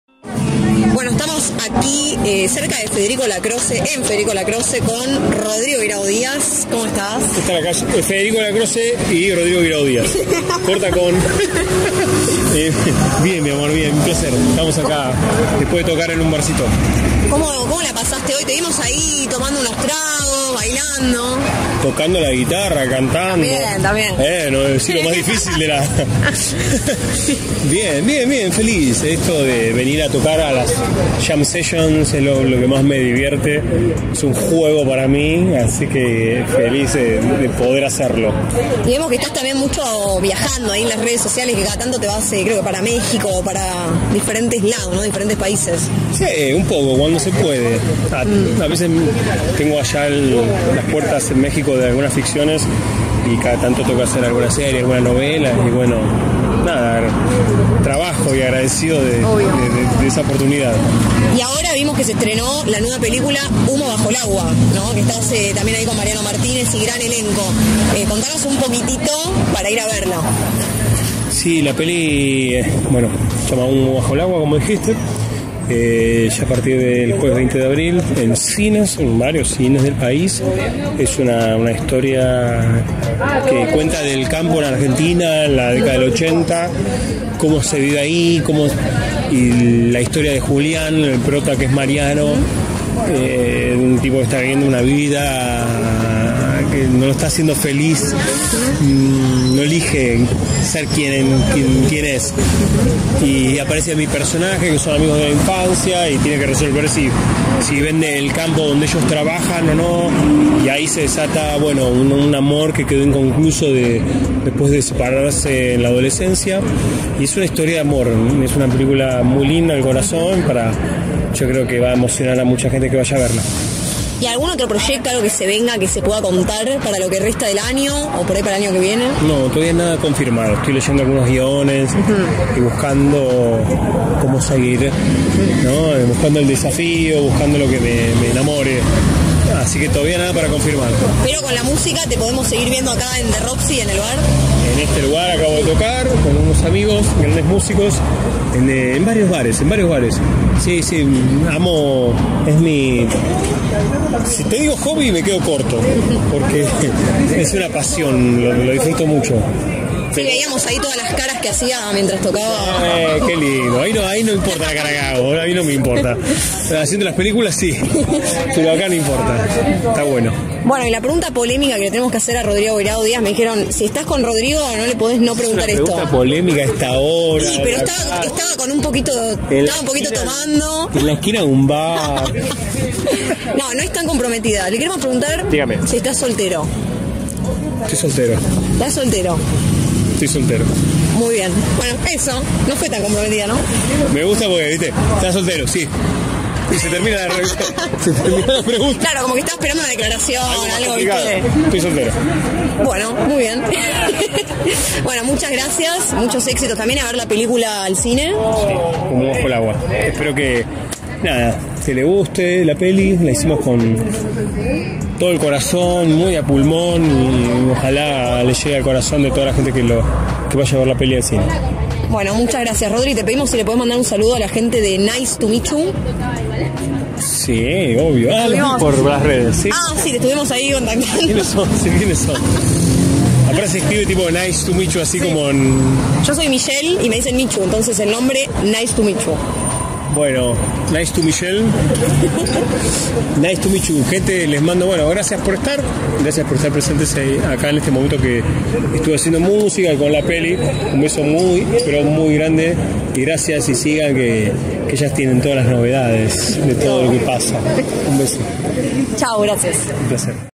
NICE TO MICHU #16 | RODRIGO GUIRAO DÍAZ
Entrevista con el actor de cine, teatro y televisión, Rodrigo Guirao Díaz.

El famoso actor y músico, Rodrigo Guirao, habló con Nice to Michu después de brindar un show en The Roxy Bar y nos contó todo sobre su nueva película ''Humo bajo el agua''.

Conducción y Producción: Michelle Fleischer
Camarógrafo y editor: Luciano Juri

LA CUEVA DE LEO